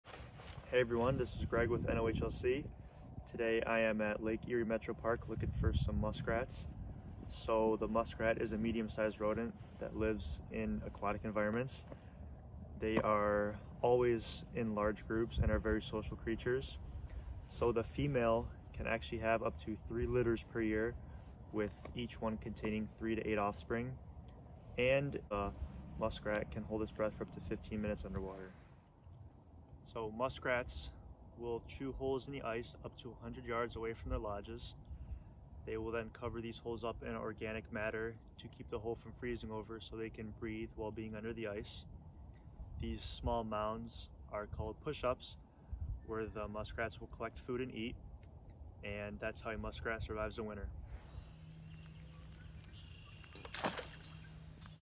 How do muskrats survive in winter?
Wandering intern Greg asks the question "How does an animal that lives in the water survive in the ice?"